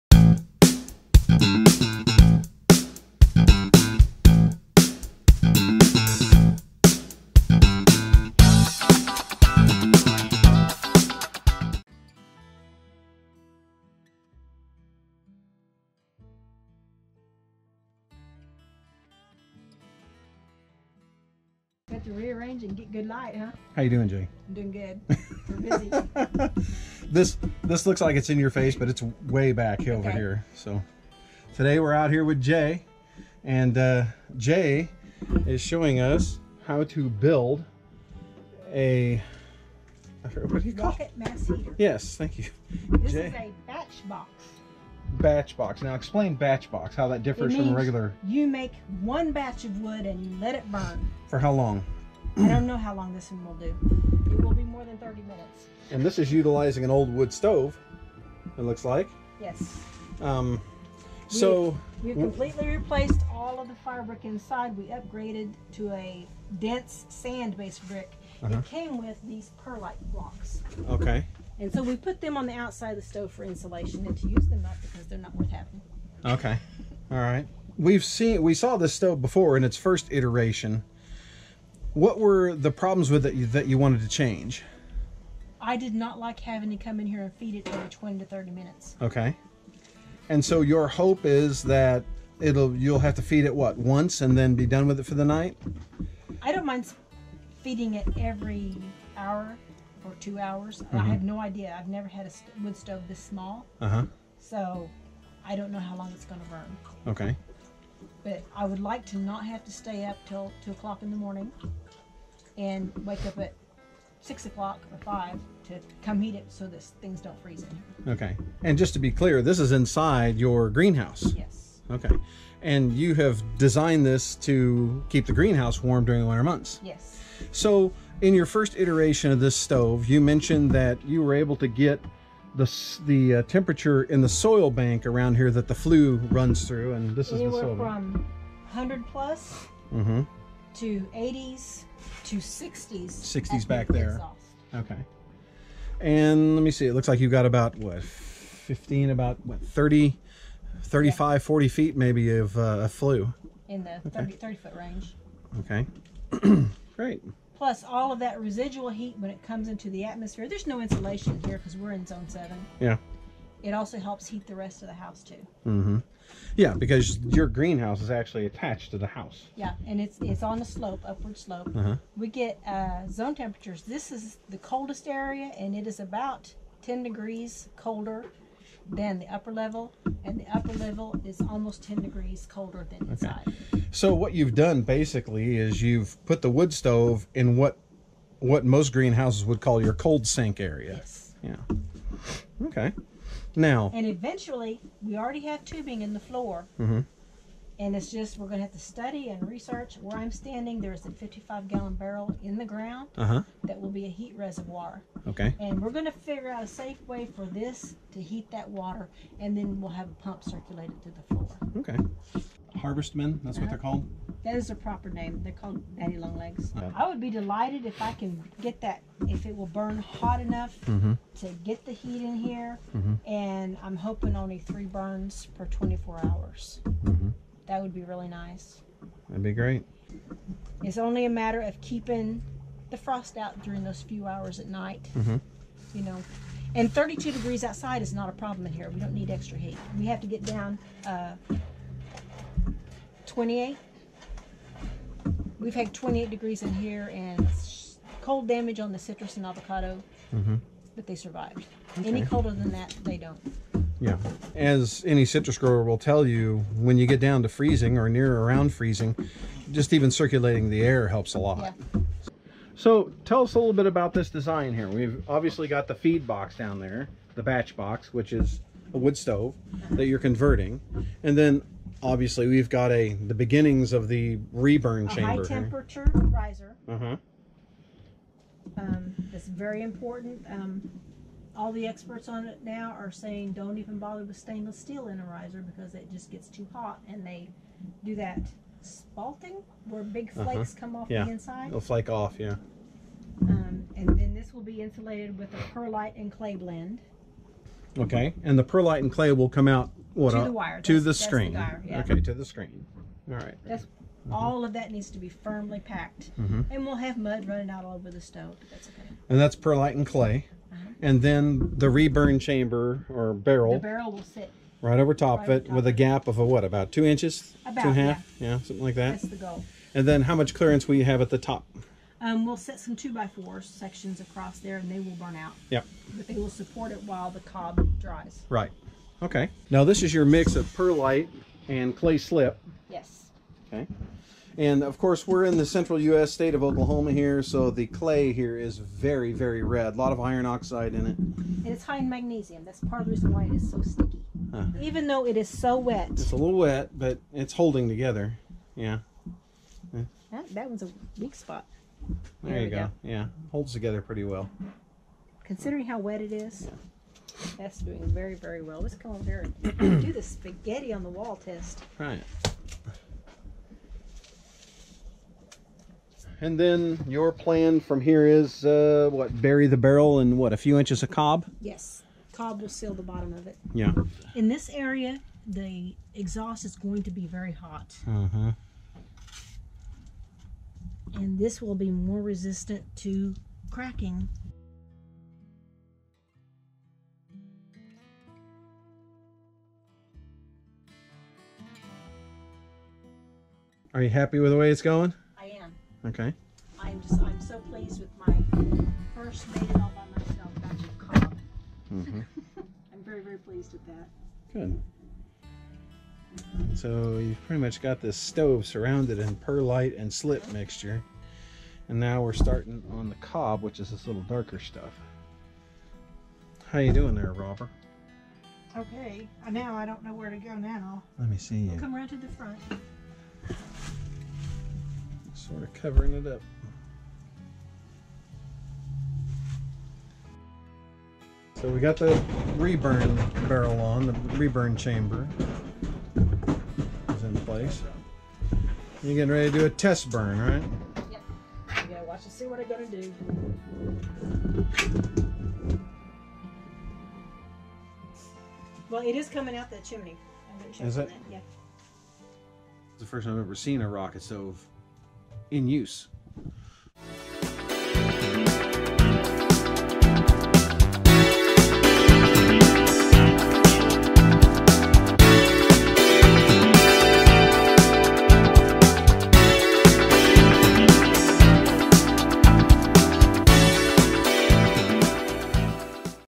Building A Rocket Mass Heater
Our off-grid living friend Jay invites us to help rebuild her rocket mass heater. This heater was built to control winter temperatures in Jay's greenhouse, but she wants to have a larger firebox in order to reduce her time feeding the fuel into it.  Jay and her husband Steve rebuild the rocket stove with a much larger firebox that should do the trick!  E-Chip and Robber had fun working in the mud and learning along the way!

e-chipandrobber